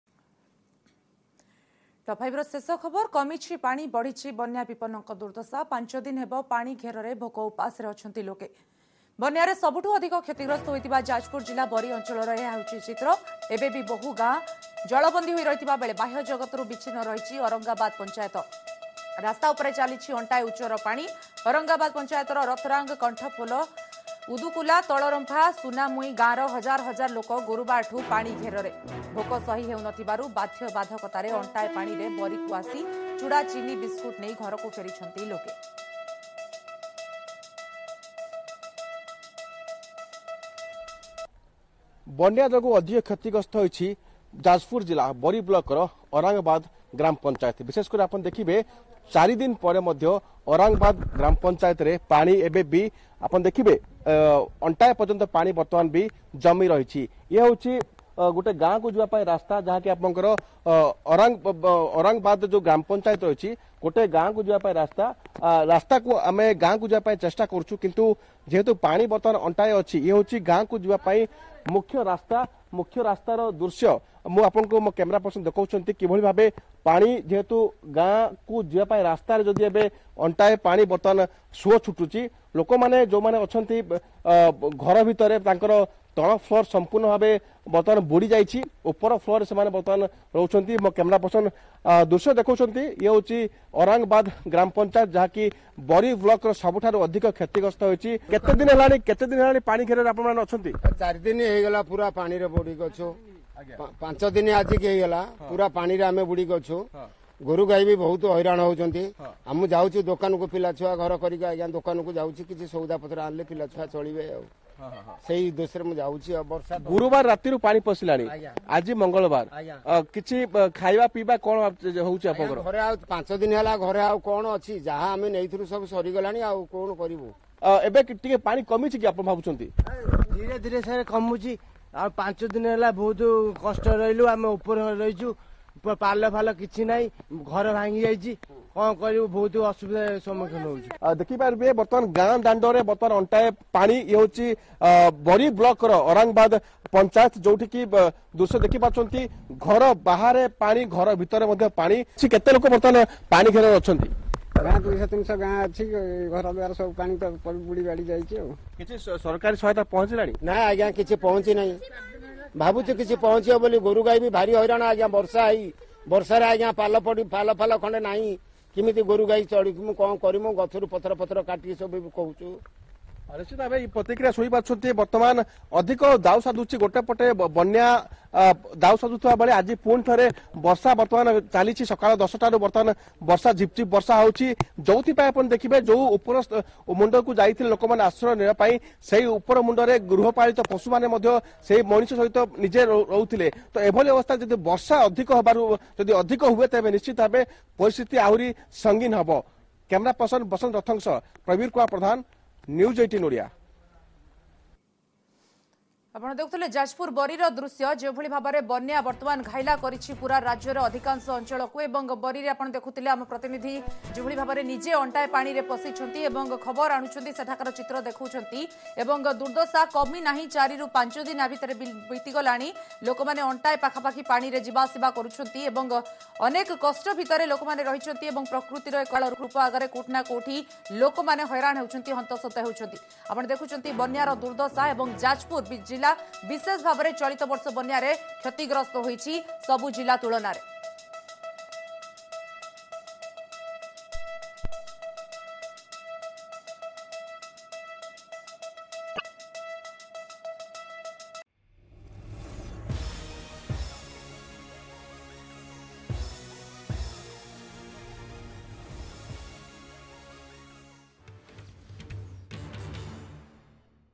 Flood Alert: ଏବେ ବି ପାଣିରେ ବୁଡ଼ି ରହିଛି ଅରଙ୍ଗବାଦ ଅଞ୍ଚଳ, ଛାତ ଉପରେ ବିତୁଛି ଜୀବନ
ଏବେ ବି ପାଣିରେ ବୁଡ଼ି ରହିଛି ଅରଙ୍ଗବାଦ ଅଞ୍ଚଳ, ଛାତ ଉପରେ ବିତୁଛି ଜୀବନ, ଅନେକ ଝାଟିମାଟି ଘର କ୍ଷତିଗ୍ରସ୍ତ I ଭୋକ ଉପାସରେ କଟୁଛି ଜୀବନ I 1st Sept 20